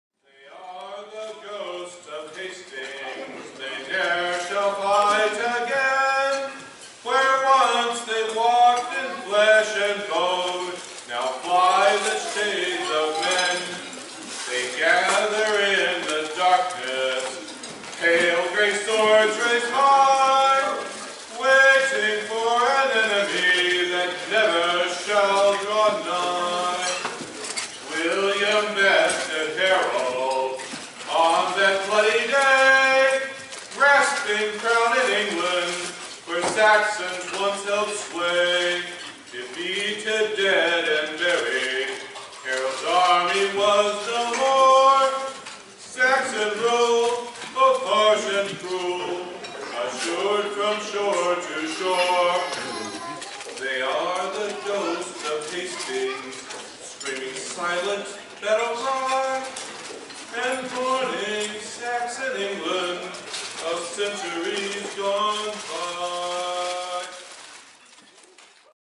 Hearkening back to Hastings -- Albrecht of Caer Anterth Mawr
Hearkening back to Hastings – sometimes one battle can change the course of history and that placename forever takes on a special meaning. This year, Bardic Madness finds itself in Hastings, Minnesota, one of at least 25 places around the world that is named after the Battle of Hastings. Present a song, story, or poem that is related to the story of the Battle of Hastings, the participants in that battle, or the consequences of that specific battle.